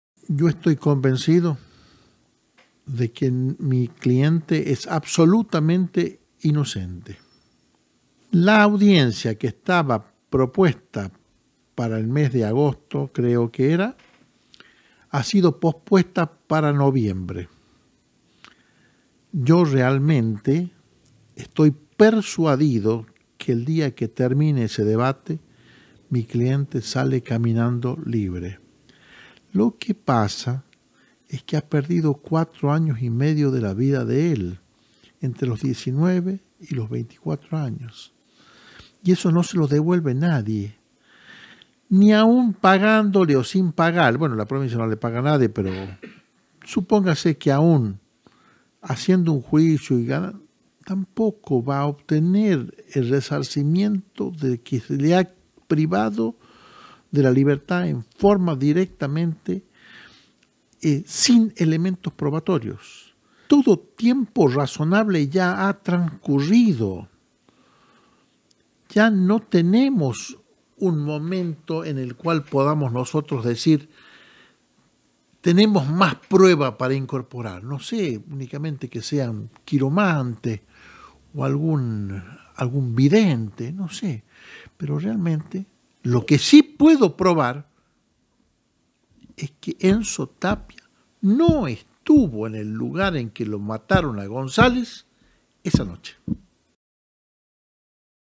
DiarioChilecito.com
Crimen de Venancio González: El abogado de Tapia, uno de los 4 detenidos por el asesinato en Nonogasta, aseguró que el día del debate su cliente saldrá libre, pero se lamentó por los 4 años que lleva detenido "sin elementos probatorios".